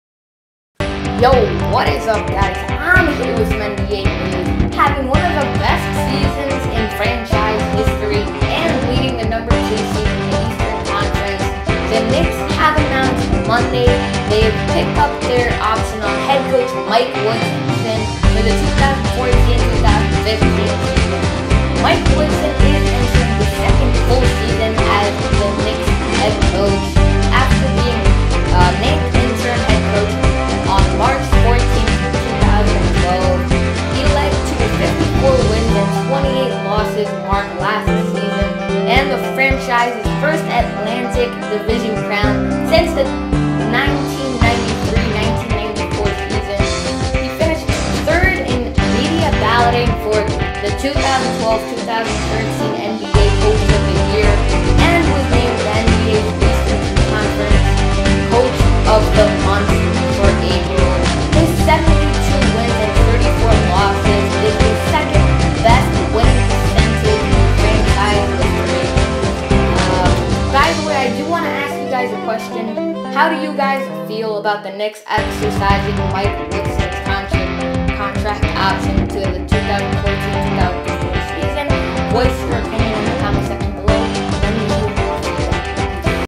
NBA- Knicks pick up option on Mike Woodson for 2014-2015!?!
How do you guys feel about this news. Do you guys think the Knicks made a good Decision in 2012?